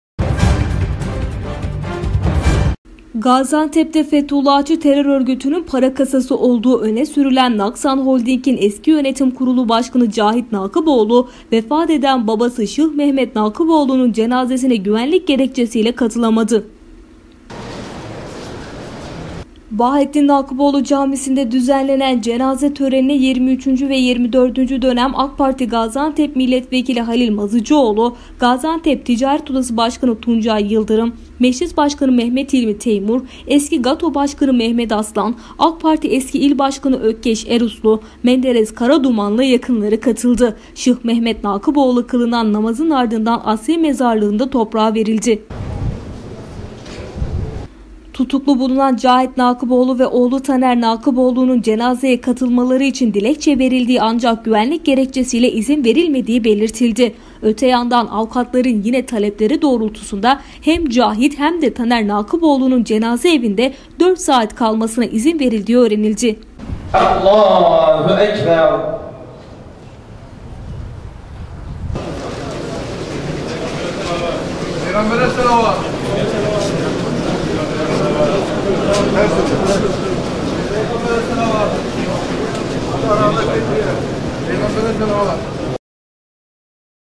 NAKIPOĞLU BABASININ CENAZESİNE KATILAMADI
Gaziantep'te Fetullahçı Terör Örgütü'nün (FETÖ) "para kasası" olduğu öne sürülen Naksan Holding'in eski yönetim kurulu başkanı Cahit Nakıboğlu vefat eden babası Şıh Mehmet Nakıboğlu'nun cenazesine güvenlik gerekçesiyle katılamadı.